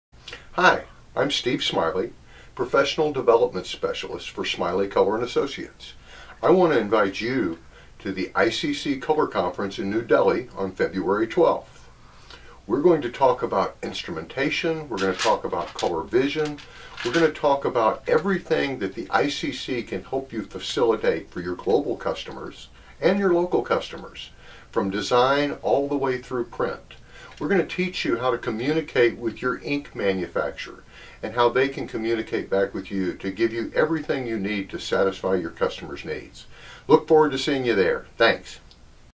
Steve Smiley to speak at NPES-ICC Color Management Conference 2015, India
Join us for this one-day conference set to take place in conjunction with the PRINTPACK INDIA 2015 show. Renowned overseas experts are invited to share the latest trends of ICC color management and the applications in different areas. You will hear and participate in discussions on color management, from capture to production.